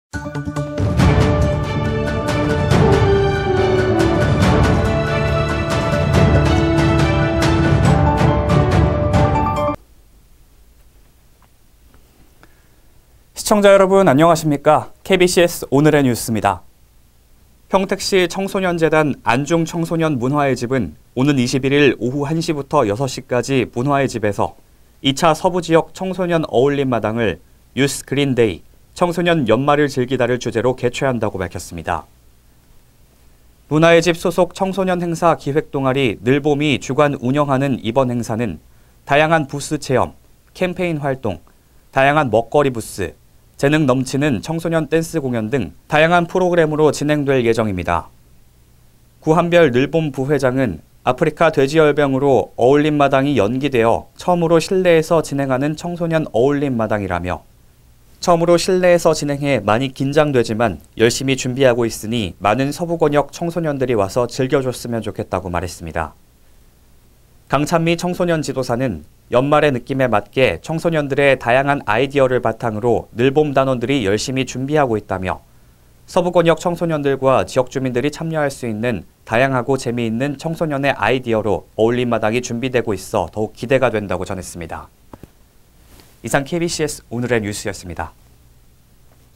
평택시, 2차 서부지역 청소년 어울림마당 청소년 연말을 즐기다
평택시청소년재단 안중청소년문화의집은 오는 21일 오후 1시부터 6시까지 문화의집에서 2차 서부지역 청소년 어울림마당을 ‘Youth Green Day – 청소년, 연말을 즐기다’를 주제로 개최한다고 밝혔다.

문화의집 소속 청소년 행사 기획동아리 ‘늘봄’이 주관 운영하는 이번 행사는 ▶다양한 부스 체험(캔들 만들기, 타로, 심폐소생술, 가면 만들기, 노래자랑, 고민약국, 페이스페인팅, 트윙클 붙임머리, 매듭인형, 미니어처, 머리핀 만들기 등), ▶캠페인활동(세계 파티문화 소개, 독립 100주년 캠페인), ▶다양한 먹거리부스 (어묵, 에이드, 소떡소떡등), ▶재능 넘치는 청소년 댄스 공연 등 다양한 프로그램으로 진행될 예정이다.

구한별(현화고/3) 늘봄 부회장은 “아프리카돼지열병으로 어울림마당이 연기되어 처음으로 실내에서 진행하는 청소년 어울림마당”이라며, “처음으로 실내에서 진행하여 많이 긴장되지만, 열심히 준비하고 있으니 많은 서부권역 청소년들이 와서 즐겨줬으면 좋겠다”고 말했다.

강찬미 청소년지도사는 “연말의 느낌에 맞게 청소년들의 다양한 아이디어를 바탕으로 ‘늘봄’ 단원들이 열심히 준비하고 있다”며 “서부권역 청소년들과 지역주민들이 참여할 수 있는 다양하고 재미있는 청소년의 아이디어로 어울림마당이 준비되어가고 있어 더욱 기대가 된다”고 전했다.